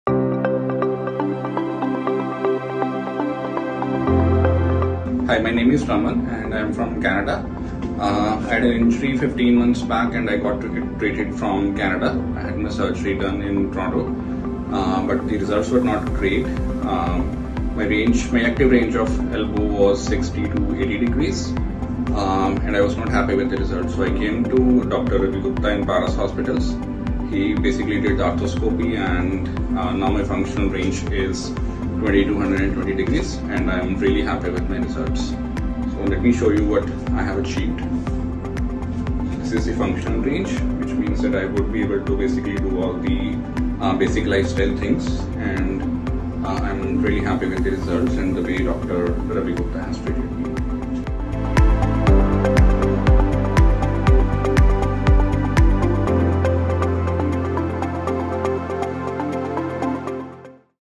International Patient | Dr. Ravi Gupta | Paras Health, Panchkula
Mr. Raman, a resident of Canada, chose Paras Health, Panchkula, for his orthopedic treatment under the expert care of Dr. Ravi Gupta, Chairman - Orthopaedics. Despite the distance, Mr. Raman's trust in our specialized healthcare services brought him to India, and he couldn't be happier with the results. His successful recovery is a testament to our commitment to excellence in patient care and advanced orthopedic solutions. At Paras Health, we continue to redefine healthcare standards, ensuring every patient receives the best possible treatment, regardless of where they come from.